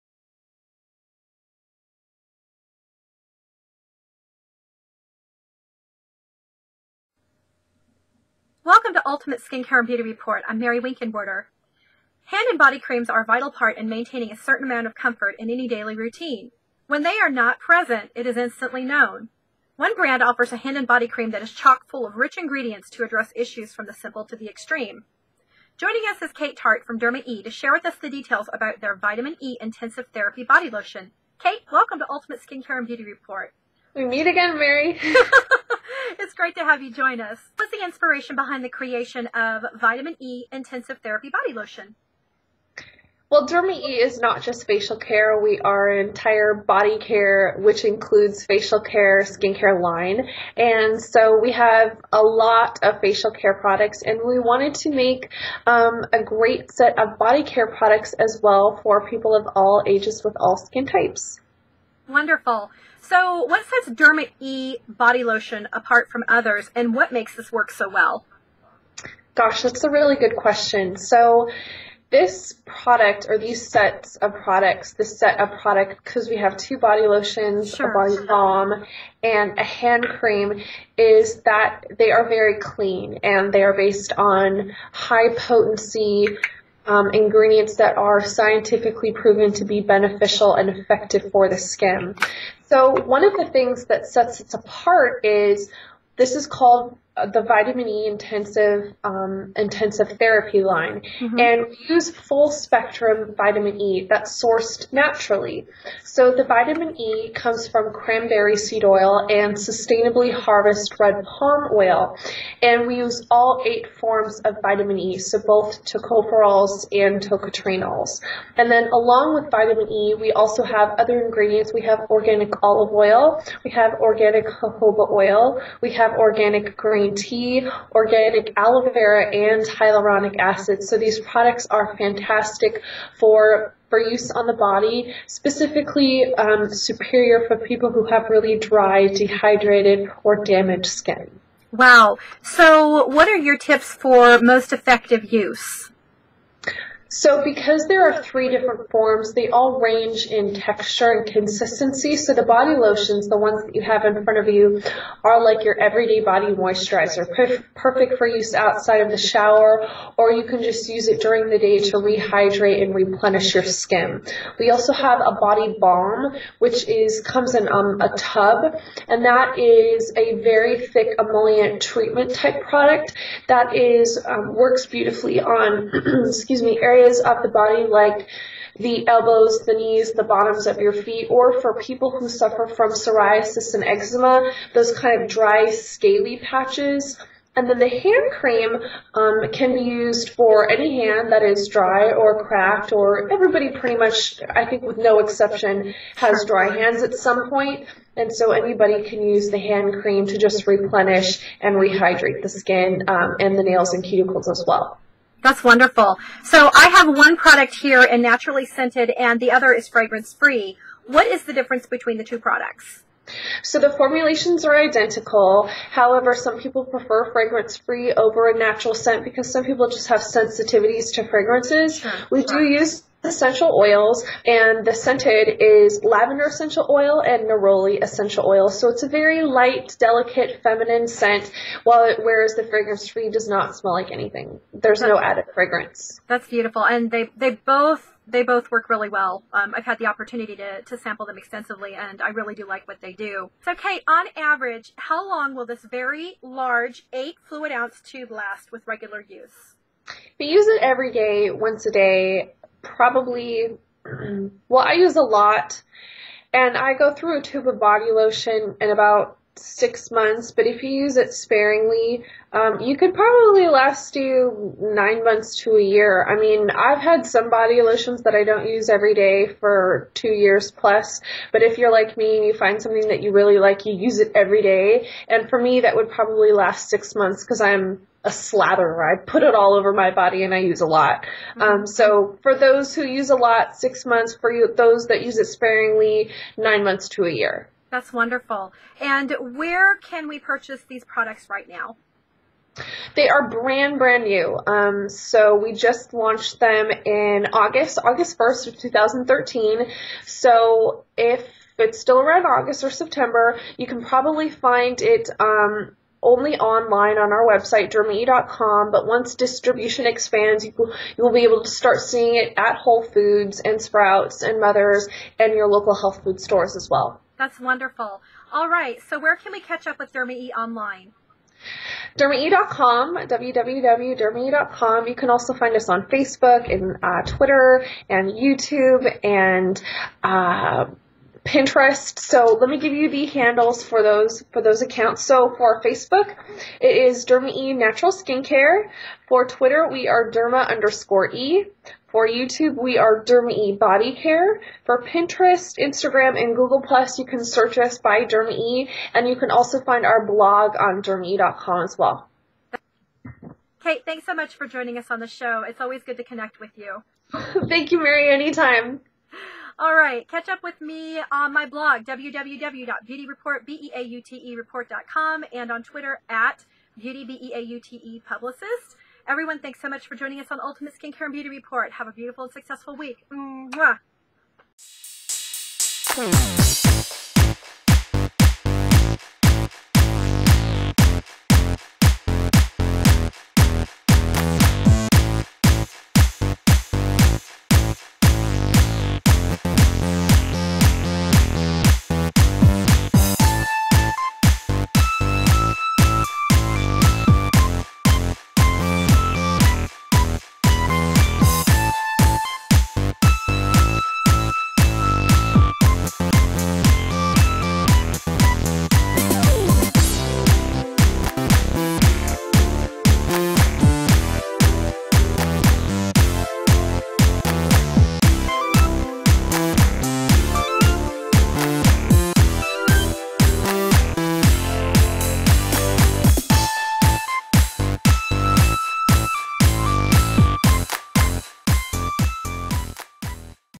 Derma e Vitamin E Intensive Therapy Body Lotion with Brand Aesthetician Kate Tart
Connecting with a reliable hand and body cream enriched with Vitamin E can be a challenge if you don't know where to look.  We connected with Kate Tart from derma e skin care and found out about their solution for keeping the hands and body supple and healthy.